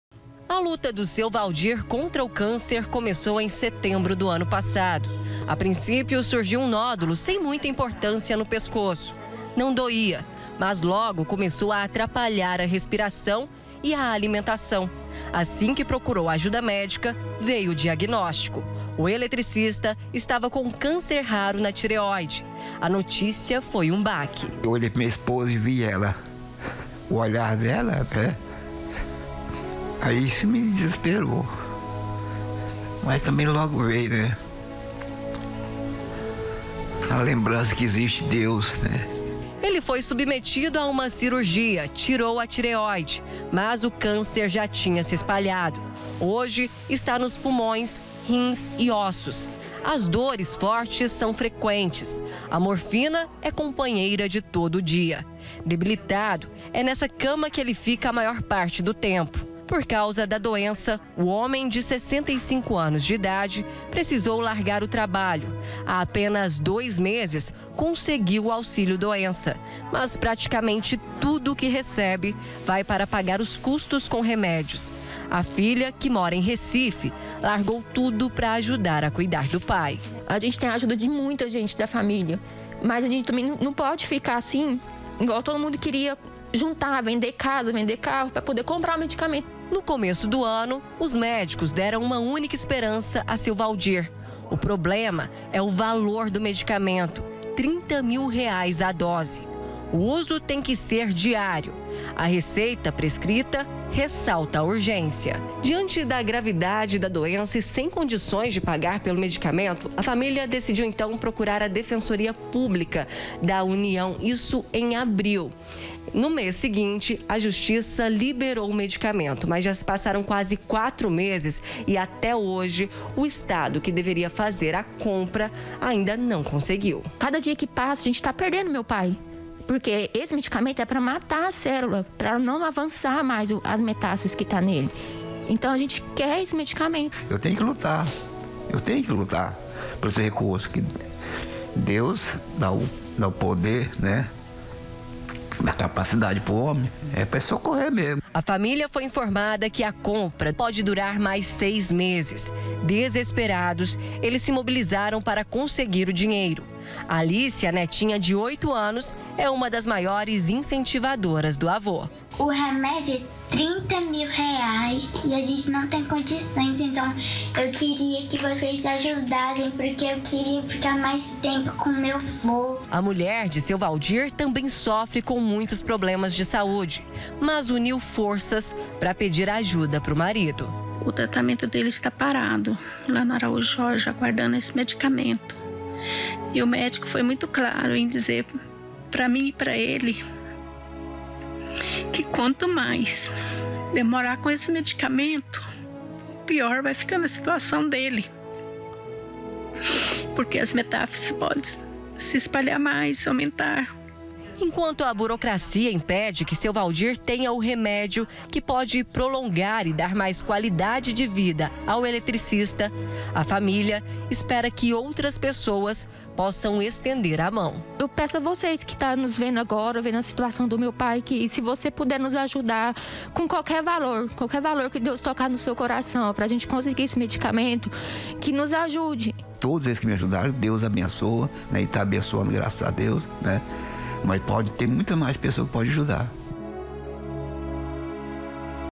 LUTA CONTRA O CÂNCER - HOMEM PRECISA DE MEDICAMENTO DE ALTO CUSTO
A FAMÍLIA DE UM SENHOR DE 65 ANOS LUTA NA JUSTIÇA PRA CONSEGUIR A MEDICAÇÃO PRA TRATAR UM CÂNCER, QUE SURGIU EM SETEMBRO DO ANO PASSADO. SEM OS REMÉDIOS A DOENÇA SE AGRAVA A CADA DIA. DESESPERADOS ELES FAZEM UM APELO PRA CONSEGUIR AMENIZAR A DOR DO HOMEM.